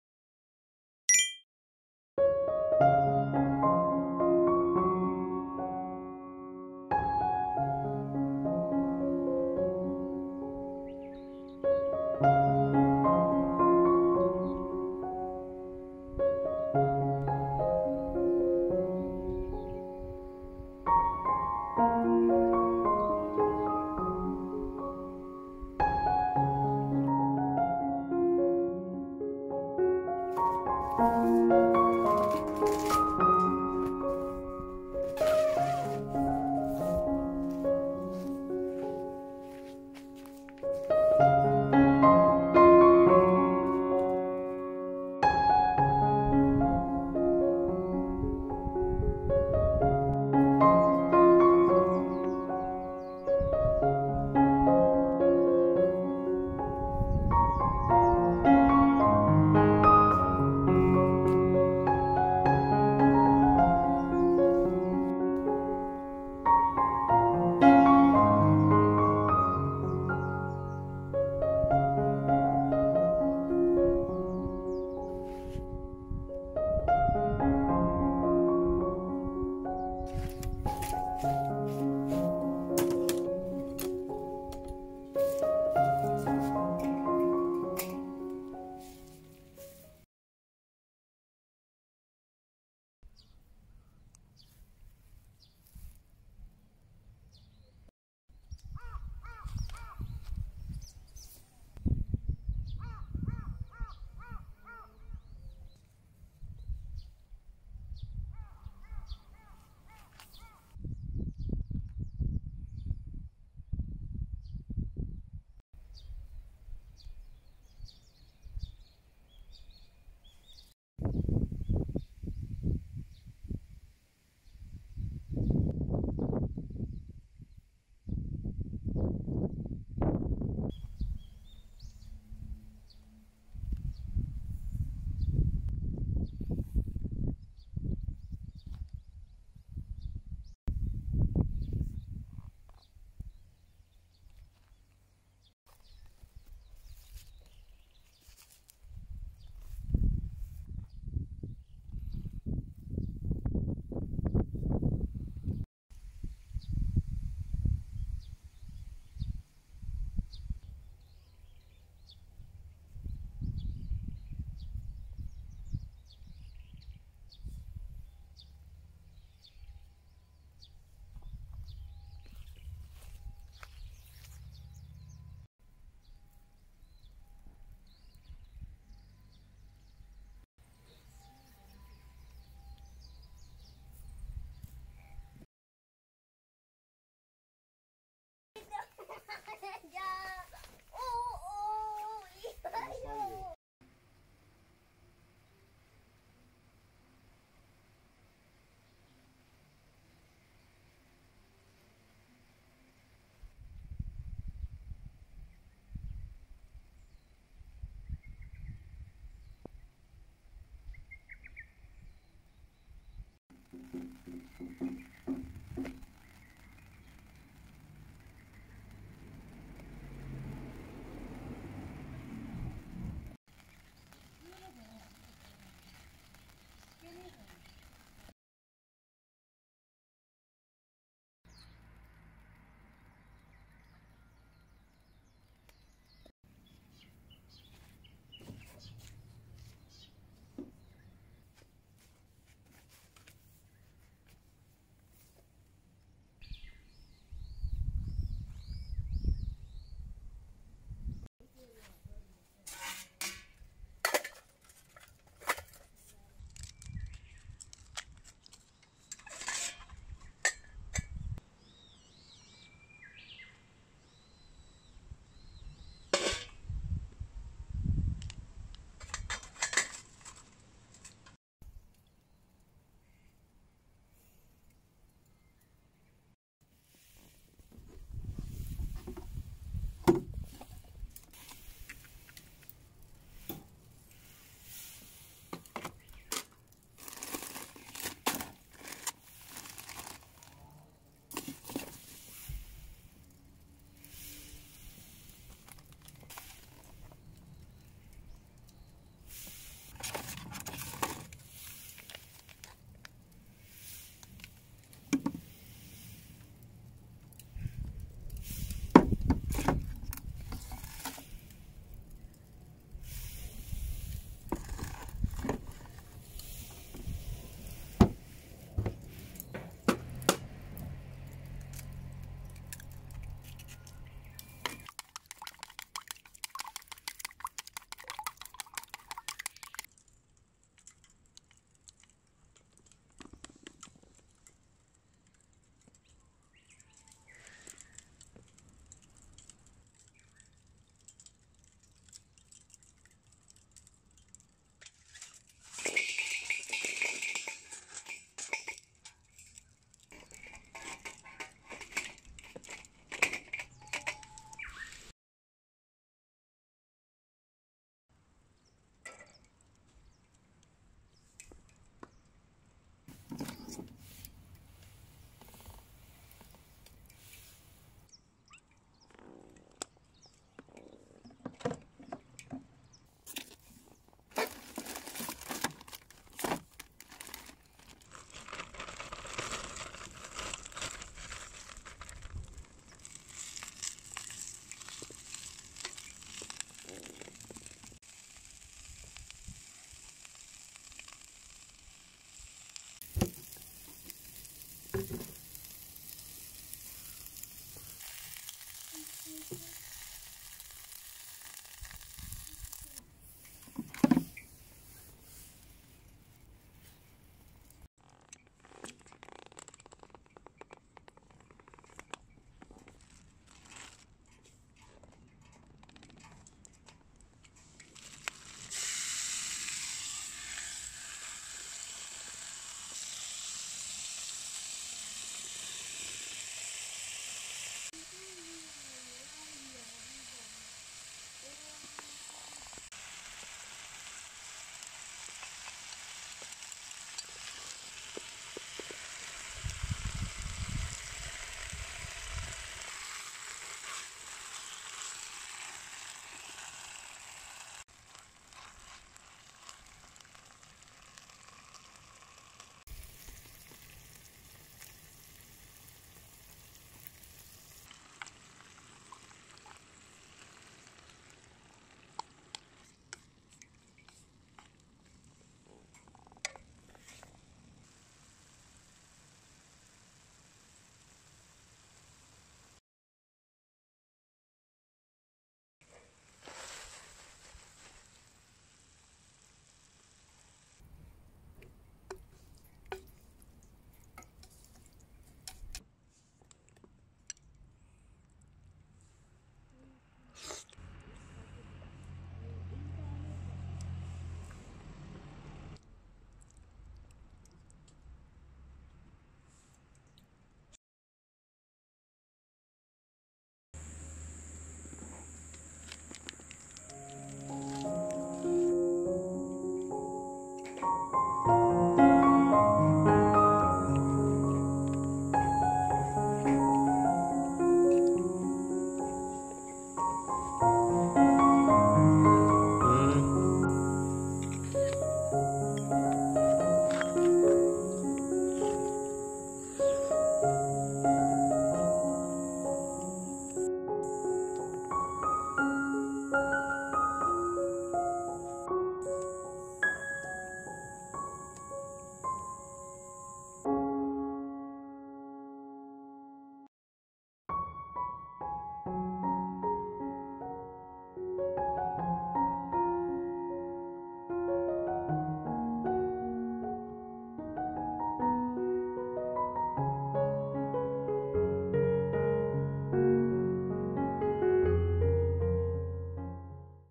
152【キャンプ】コールマン(Coleman) テント ツーリングドームST+と夜の庭キャンプ
こんにちは！休日やることです。

Hello! I will introduce what to do on holidays.

家の庭で夜キャンプします。
テントはコールマン ツーリングドームST+です。
天気は曇りです。湿った風が吹いています。

Camp at night in the home garden.
The tent is Coleman Touring Dome ST+.
The weather is cloudy. A damp wind is blowing.

▲テント
コールマン ツーリングドーム/ST+

新しい動画をお知らせするため、

ブログ始めました(*'ω'*)
文字と画像で見たい方は、こちらから閲覧できます。

本動画のブログはこちらから閲覧できます。